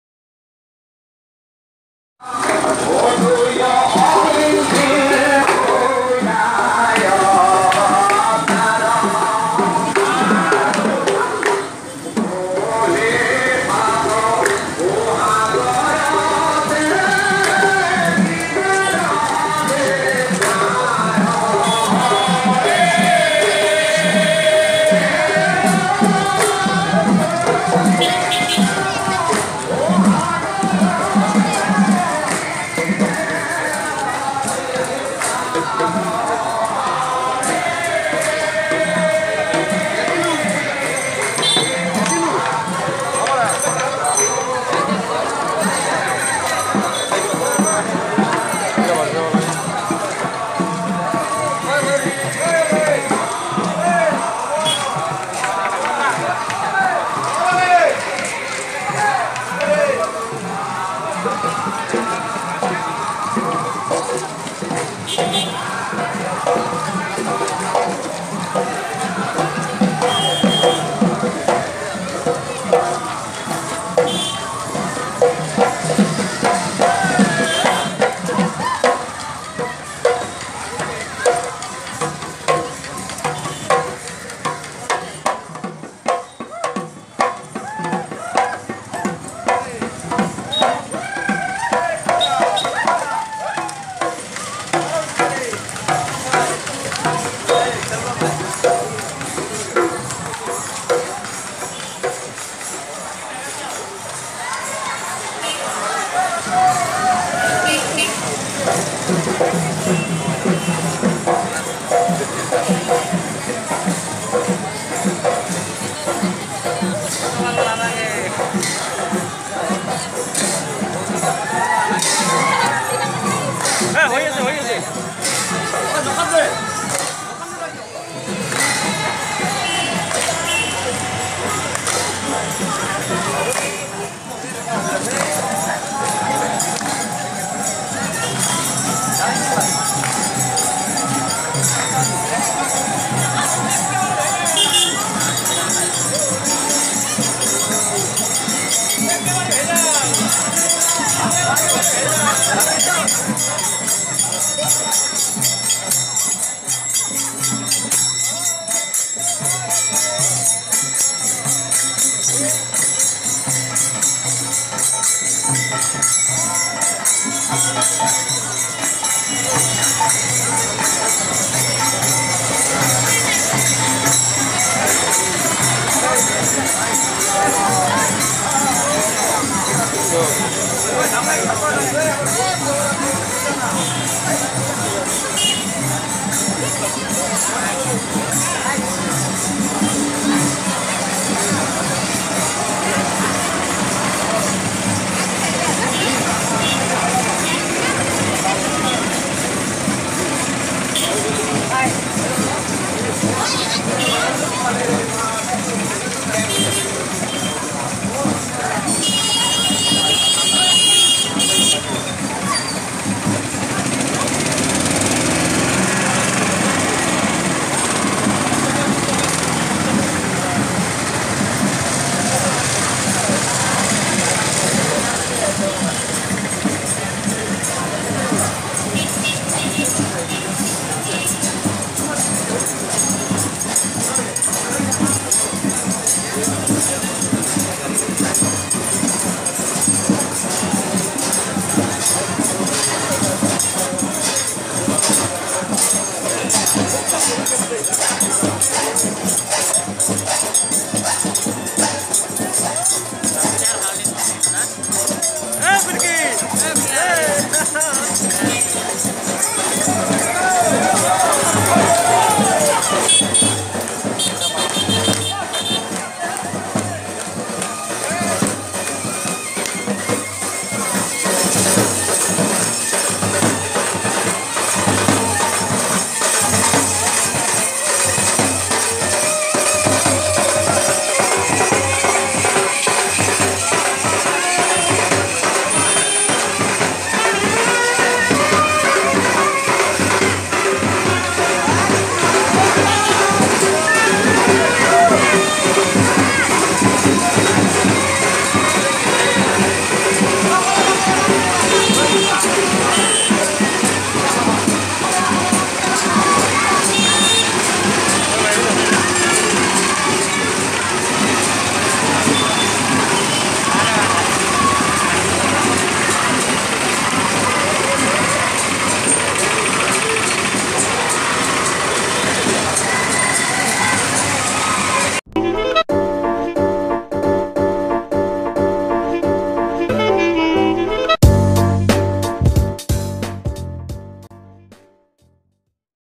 Abhayapuri Cultural Rally | Golden Jubilee Celebration of Jyoti Sangkritik Anusthan
Golden Jubilee Celebration rally at Abhayapuri, Bongaigaon.
All communities and religions are participated this this rally.

If you like this video then please like👍, Comment and share.